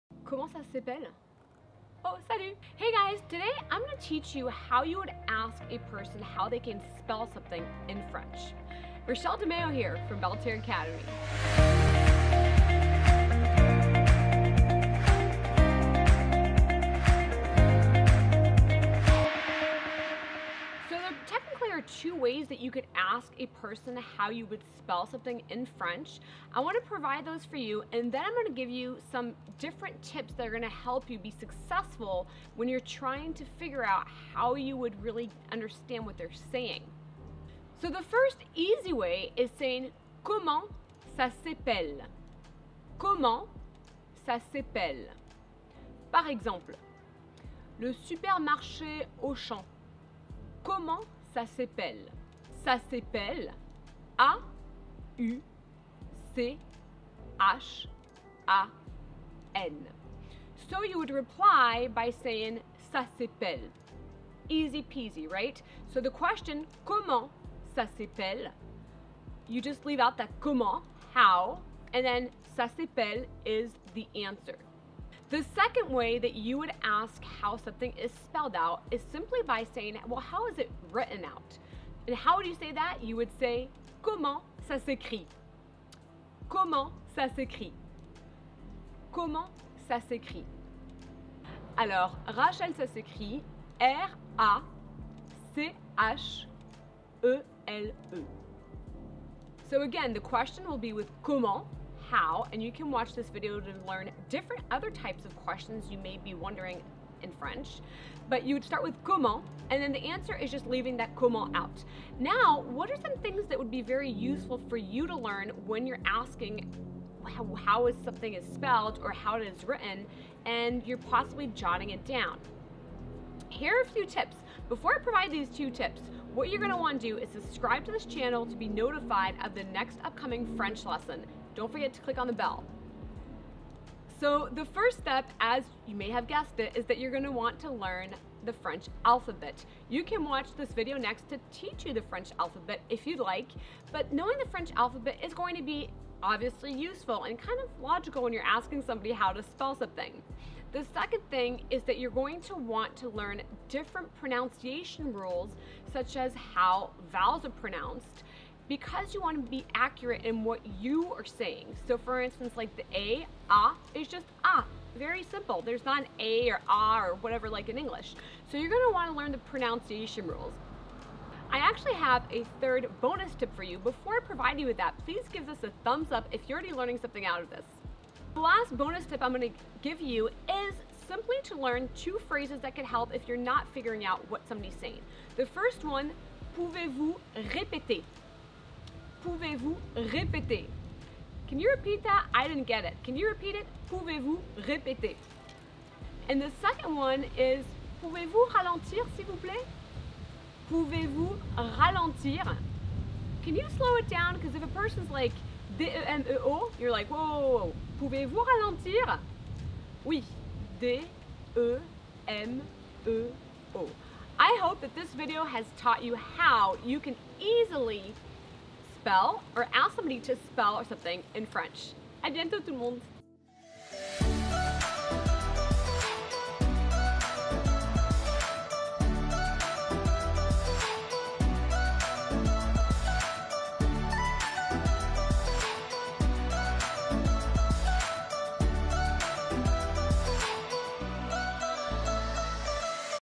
HOW to ASK SOMEONE to SPELL something in FRENCH
This French lesson teaches you ways for asking someone to spell something in French. You'll learn how to ask a person how to spell or write something in French. Native French instructor, Rachele DeMeo, from Belle Terre Academy, will also teach you how to ask someone to repeat something in French and how to ask someone to talk more slowly in French. You'll additionally discover two useful tips (as well as an extra bonus tip) so you can perfect both your French listening comprehension and your speaking skills in French.